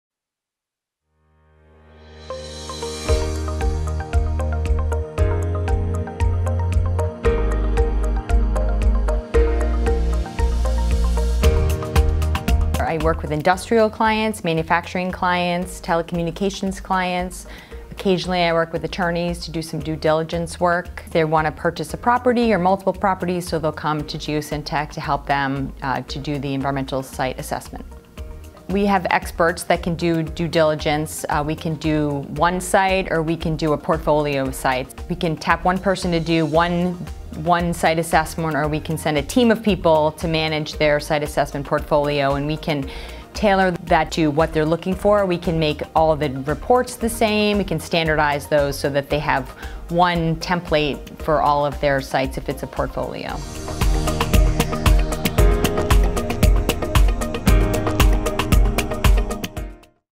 Environmental Due Diligence: Tailored Solutions for Any Number of Sites | Jenna Newcombe
As part of our commitment to environmental due diligence, Geosyntec offers tailored Environmental Site Assessments for clients.